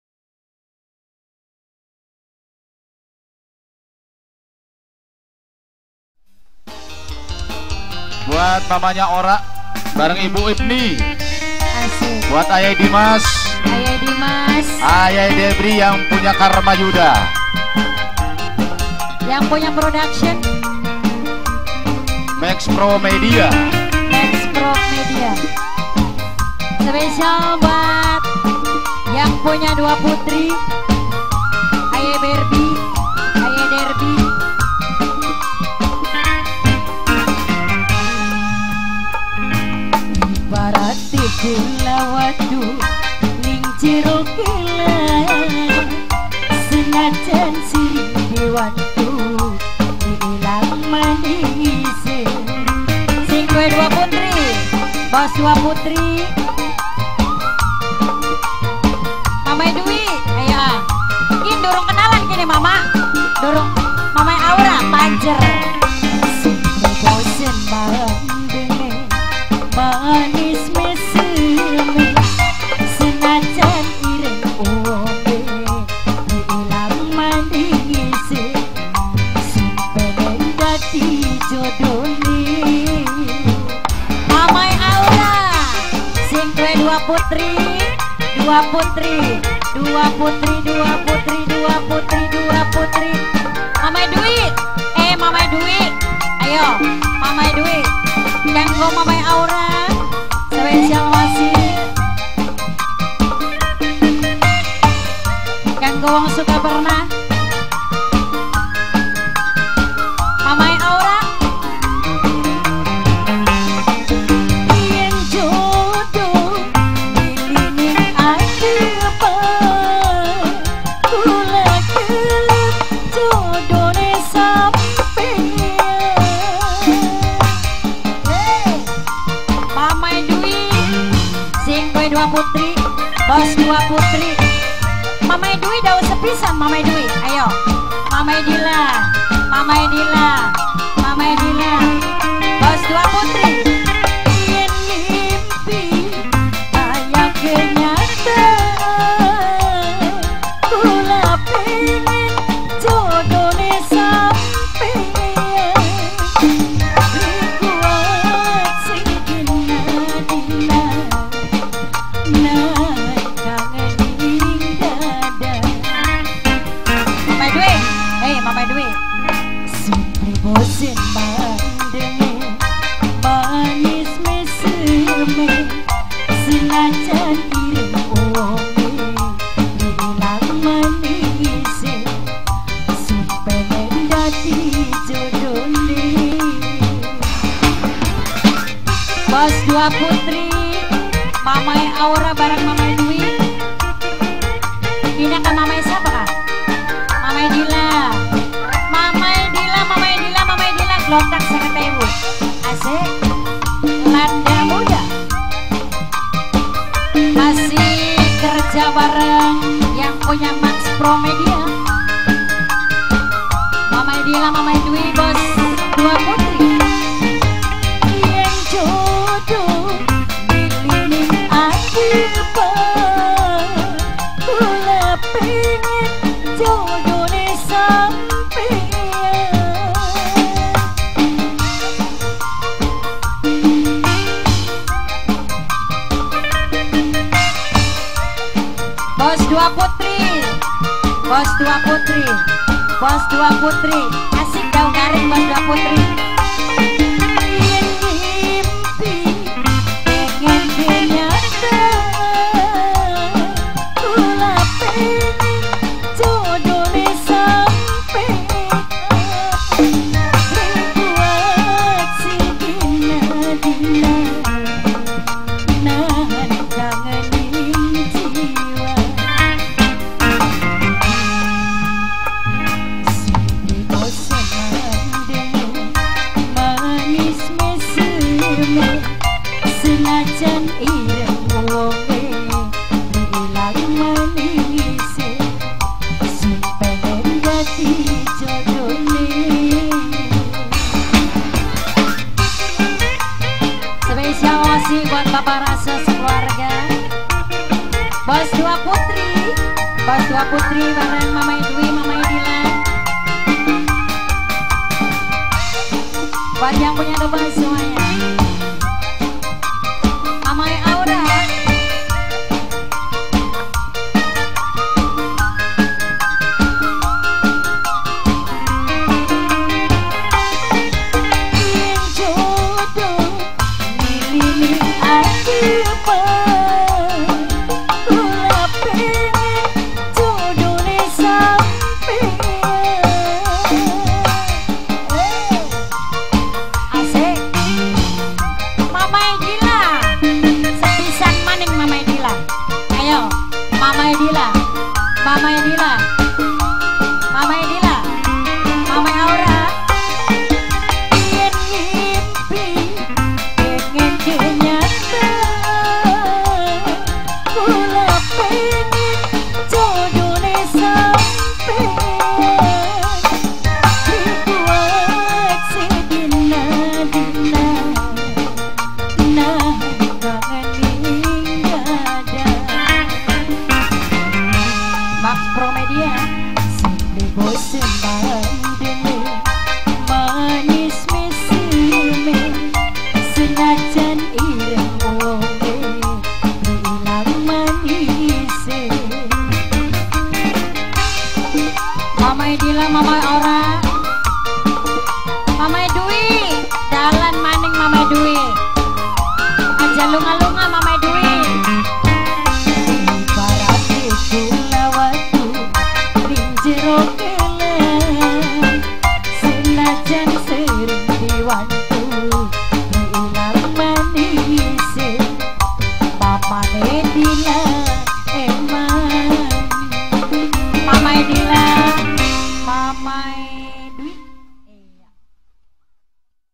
GULA WATU [ LANDA MUDA ] NGARAK 3 //9 Juni 2019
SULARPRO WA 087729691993
Resepsi hajat Bpk. Rasa / Ibu Tuminah  Dalam rangka syukuran Khitanan Muhammad Faisal dan Rasulan Hibni Noer Maulida  yang dilaksanakan pada hari Senin 10 Juni 2019  Tempat : Desa Sukaperna Rt. 07 / 03  Blok Karang Anyar Kec. Tukdana - Indramayu